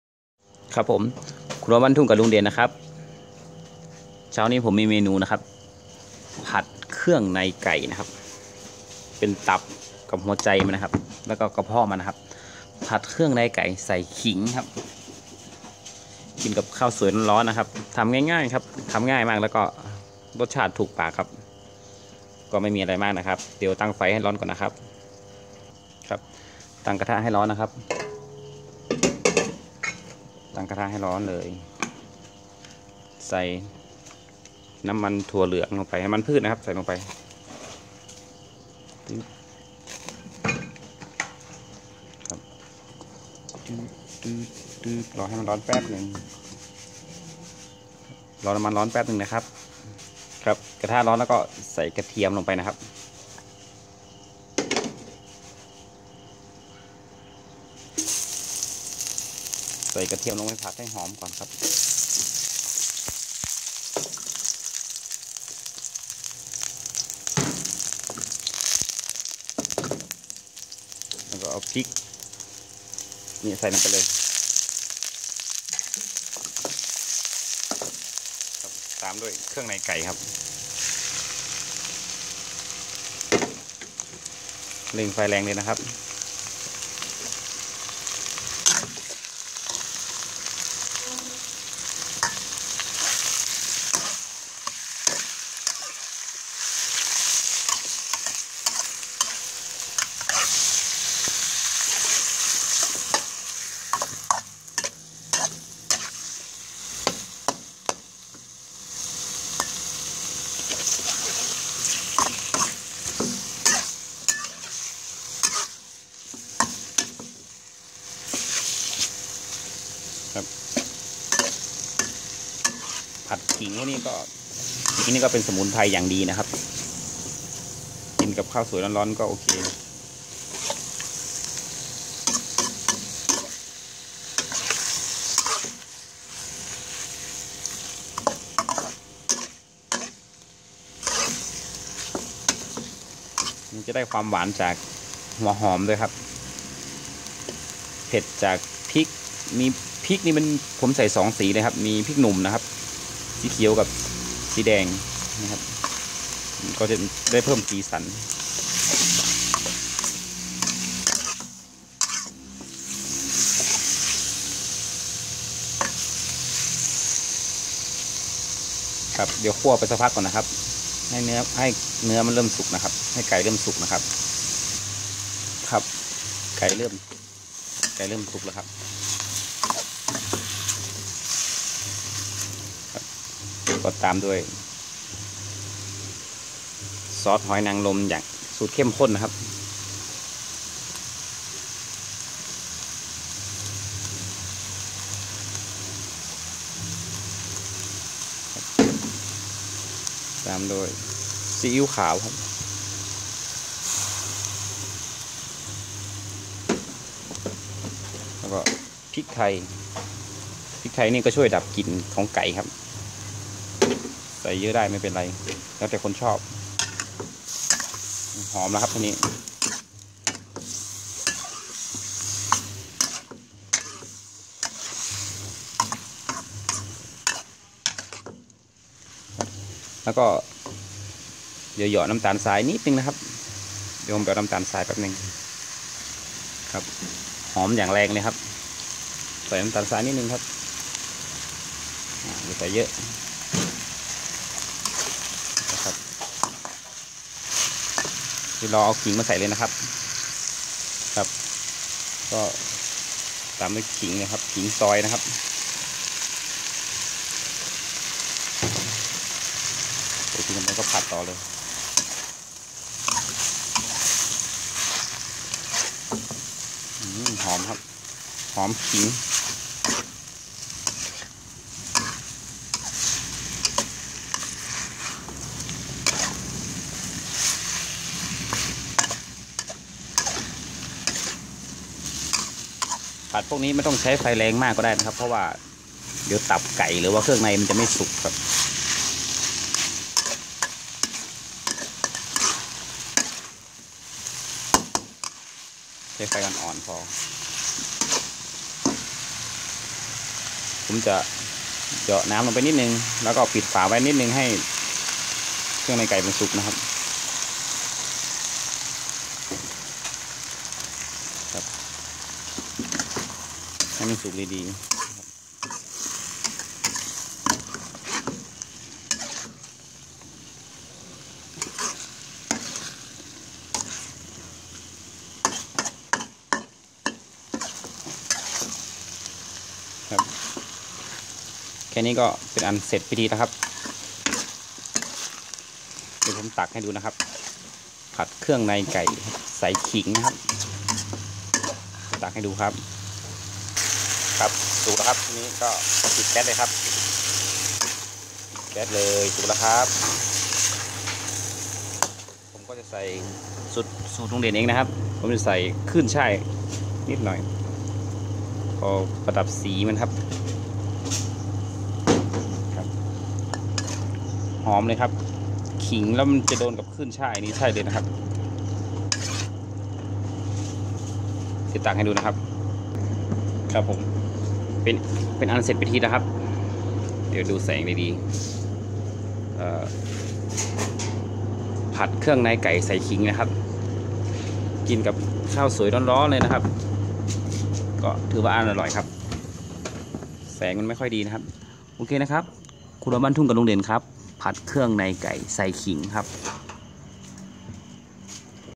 เครื่องในไก่ผัดขิง
ครัวบ้านทุ่งกับลุงเด่น พากินพาเที่ยวครับ วันนี้ลุงเด่น จะพามาทำอาหารครับ เมณู เครื่องในไก่ผิดขิง กินกับข้าวสวยร้อนๆ อาหารพื่อสุขภาพครับ